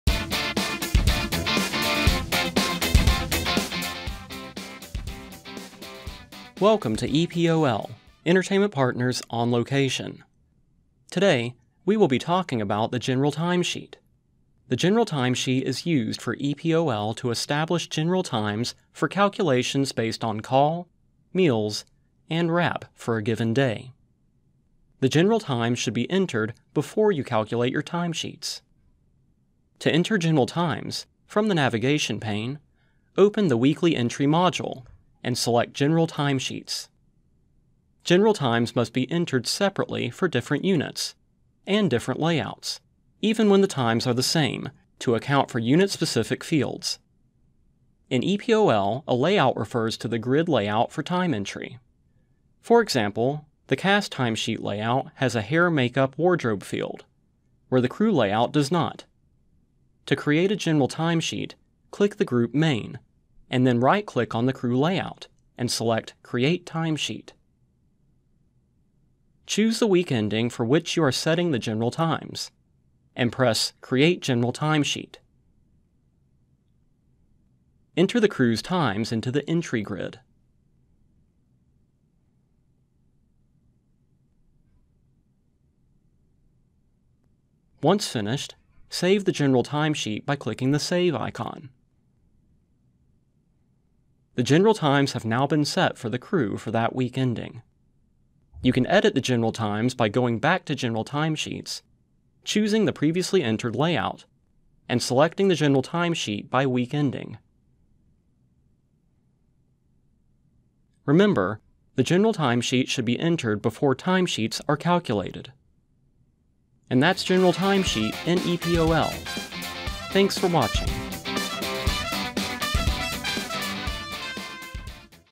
EPOL - General Timesheet
How to navigate and use the General Timesheet using EPOL